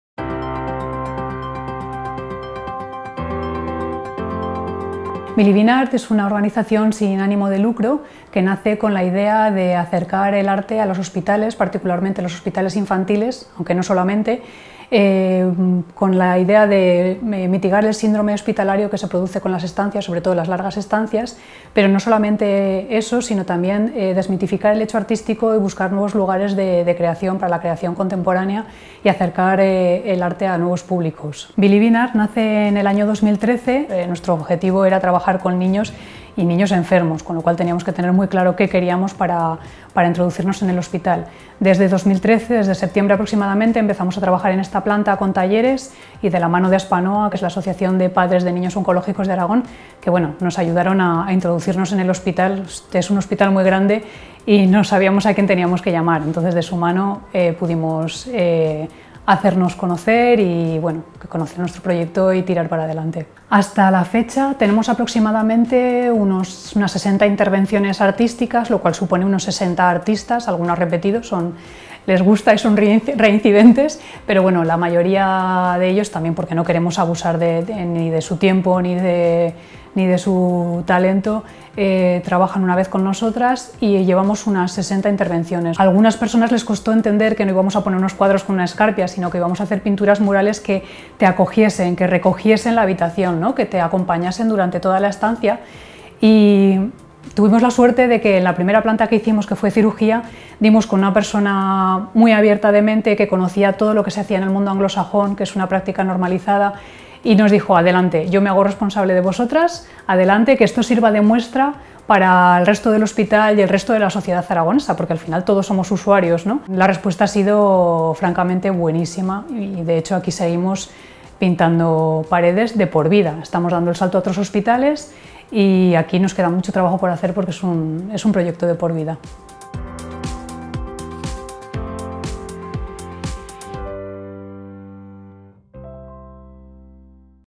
Proyecto Believe in Art - zinkinn.es
Believe in Art nace con vocación de permanencia como organización sin ánimo de lucro con el fin primero de mitigar el síndrome hospitalario y mejorar la estancia de pacientes y familiares, así como de los trabajadores, a través de la creación contemporánea.